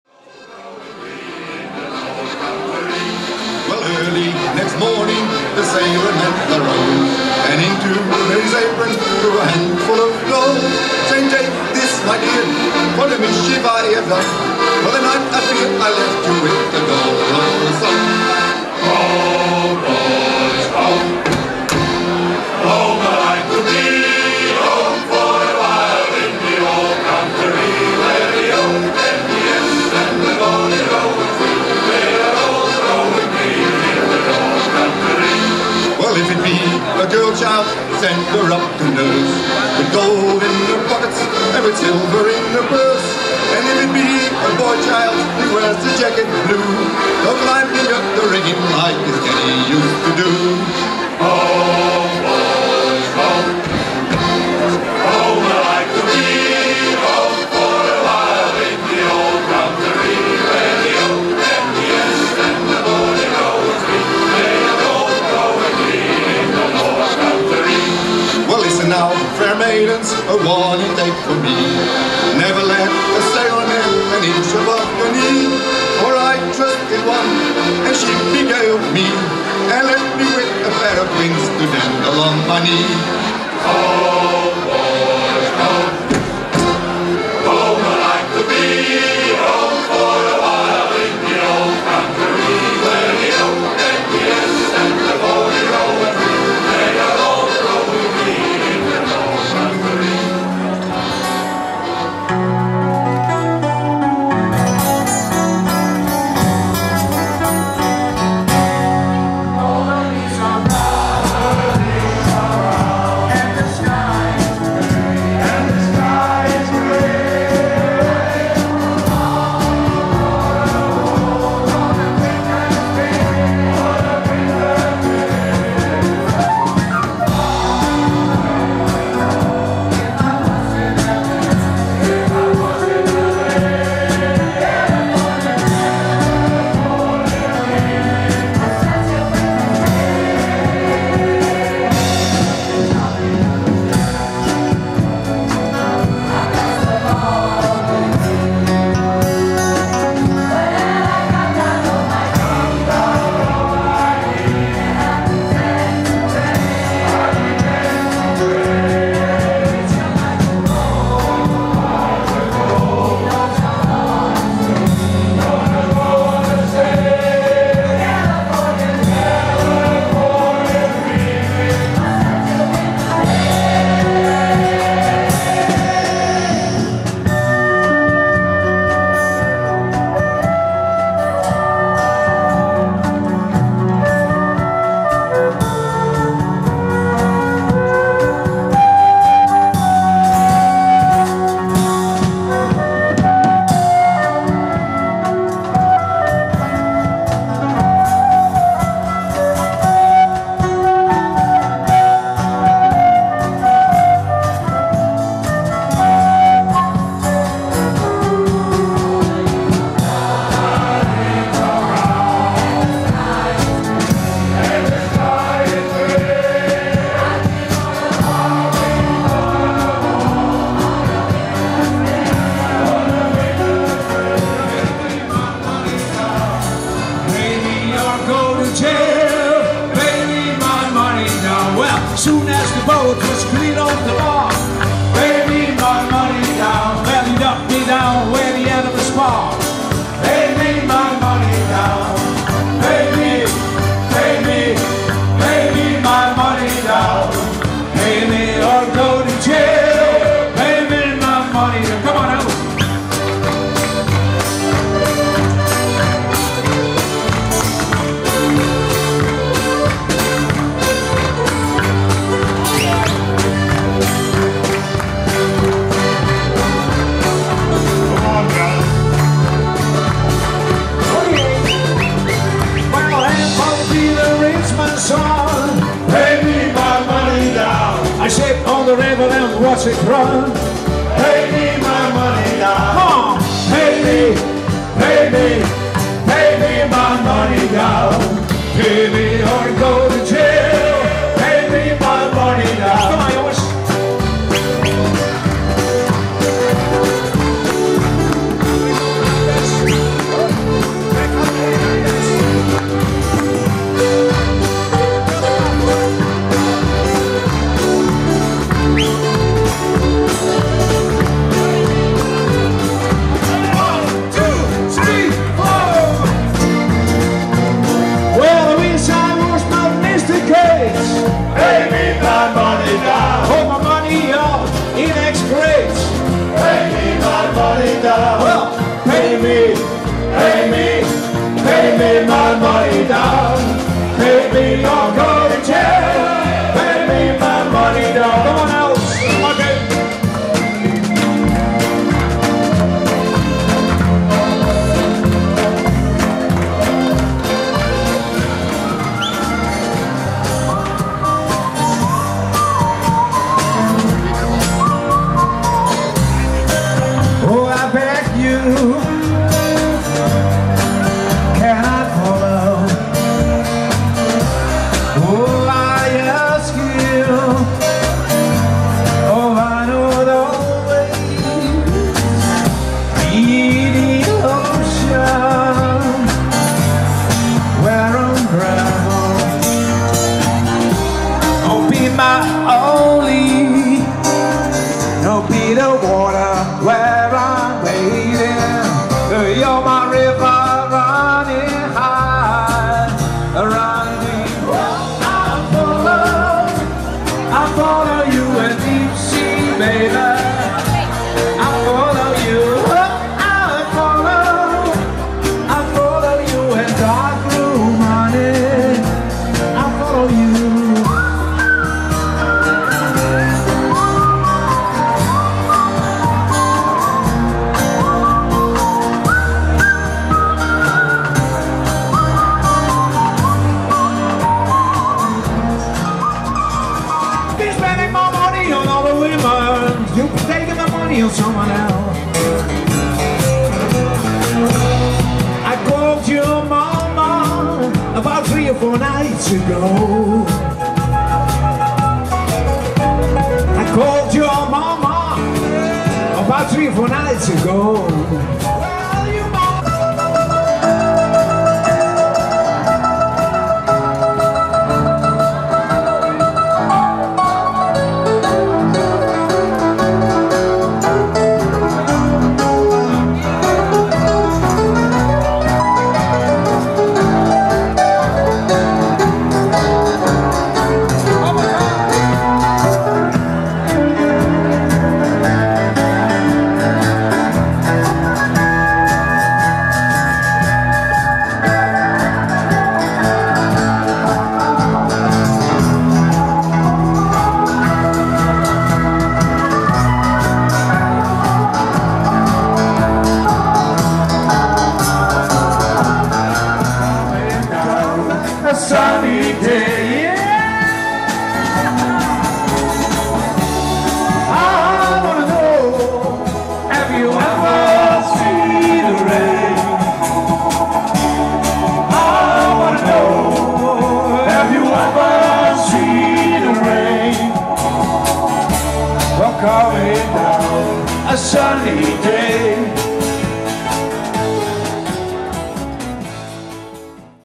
Poker Limbeats Skulpers
Een avond met optreden van Poker voorheen The Frogs Ltd en
   De Limbeats en De Skulpers in De Burgerij in Limmen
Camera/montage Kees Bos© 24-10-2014